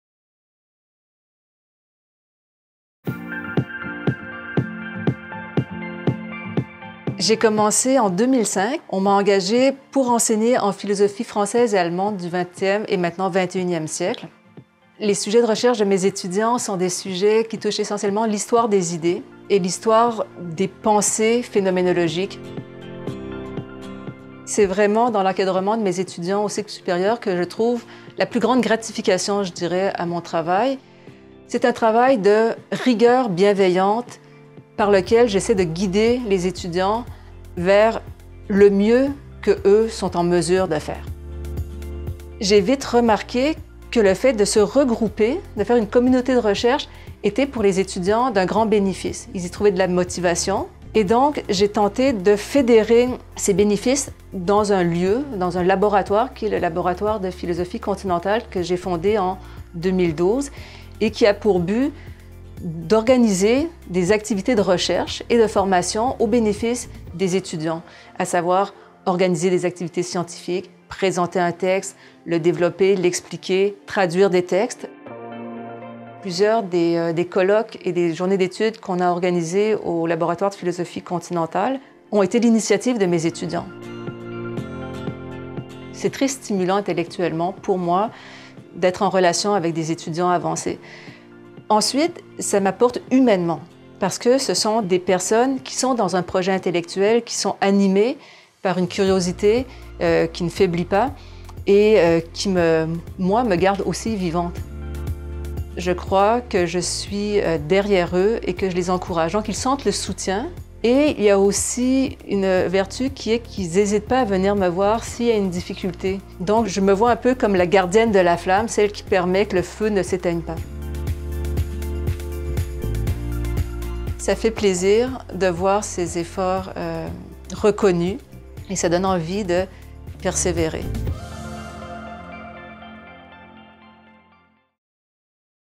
Lauréate : Sophie-Jan Arrien
Dans le cadre des Prix d'excellence en enseignement 2018, un prix Encadrement aux cycles supérieurs a été décerné à madame Sophie-Jan Arrien, professeure titulaire à la Faculté de philosophie. Professeure au Département de philosophie depuis 2005, elle oeuvre dans le champ de la philosophie continentale et, plus spécifiquement, dans l’approche phénoménologique. Soucieuse d’offrir un encadrement de qualité, madame Arrien est une directrice de recherche rigoureuse, engagée et disponible.